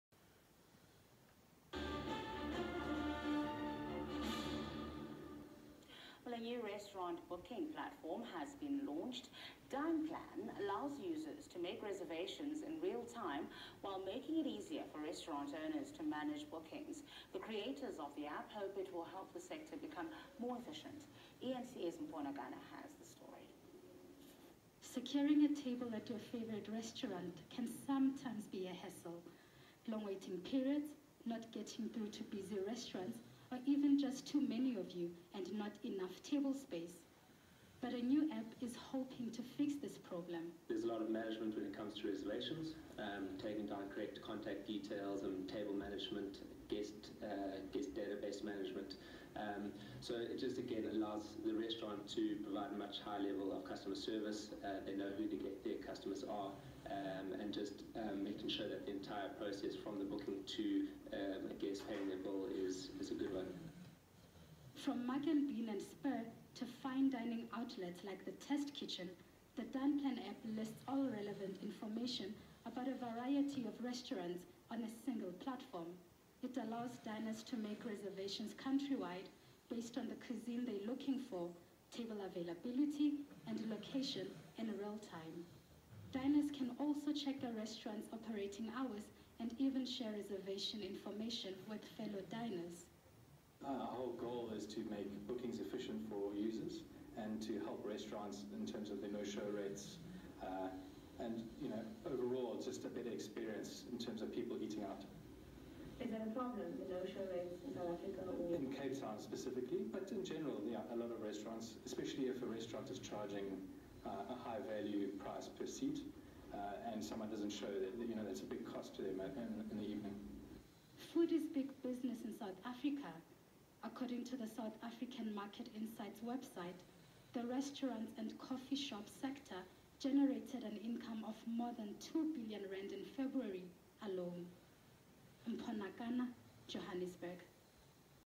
Dineplan App eNCA Interview
Watch the eNCA NEWS interview in our Bluegrass Cape Town office with the Dineplan founders. This is the phenomenal new real-time restaurant booking app that is taking the industry and South Africa by storm.